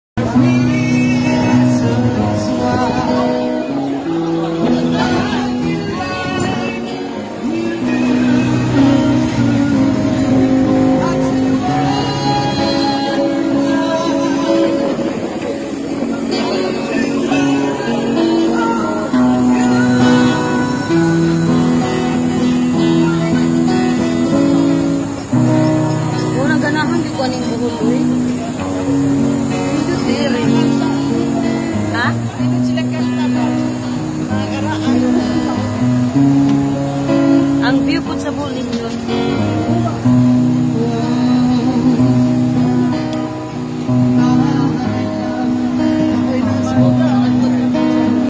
Vlog#06 A short visit in Bohol in 2014!
We had a very short visit in Bohol just to attend the birthday party of a friend who happens to have had vacation to her parents place. I am with the company of a good friend, a high school batchmate of mine. I Love Bohol!